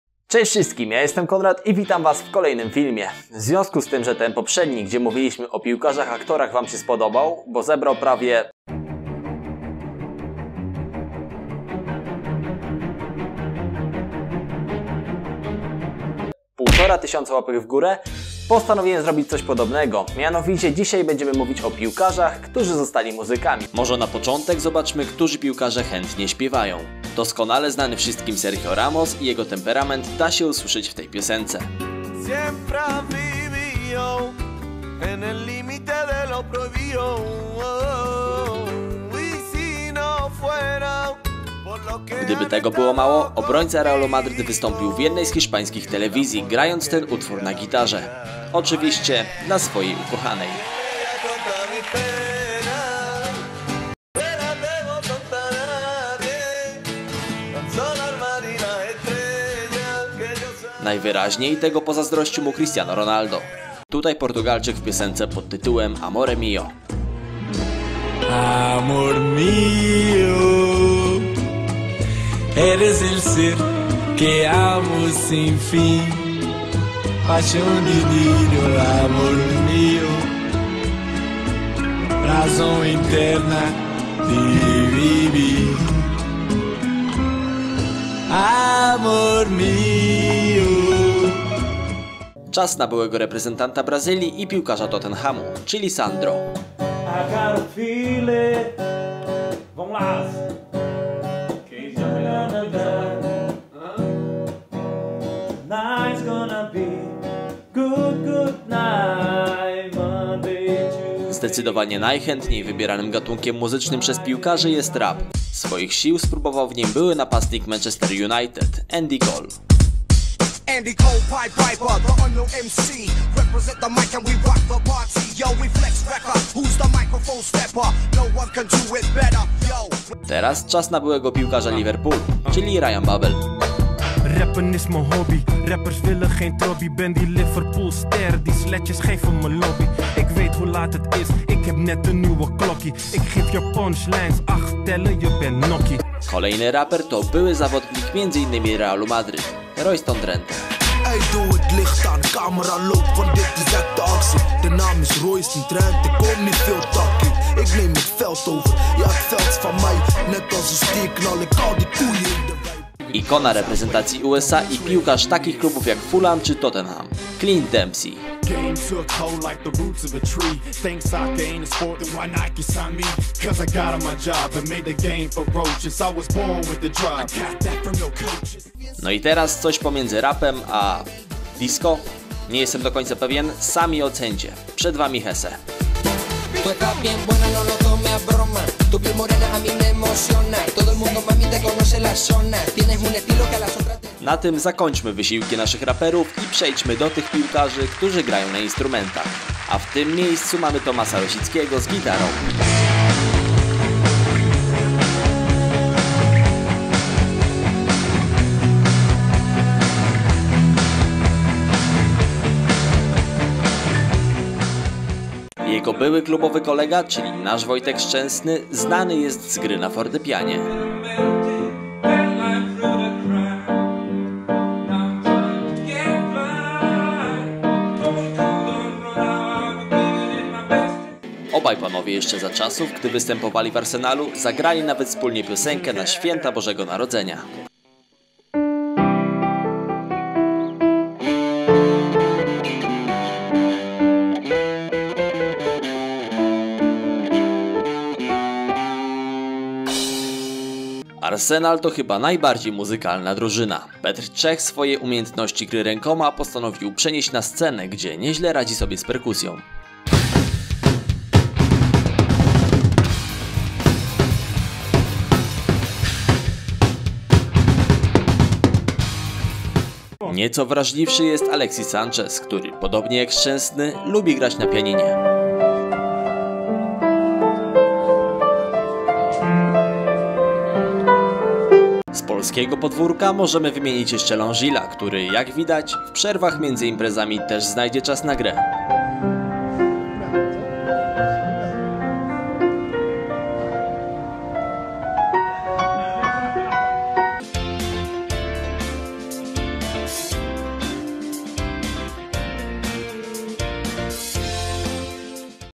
Piłkarze, którzy zostali MUZYKAMI
Tym razem przyjrzymy się piłkarzom, którzy swoją pasję odnaleźli w muzyce.

Zajrzyj na mój kanał, gdzie znajdziesz więcej filmów o piłce nożnej!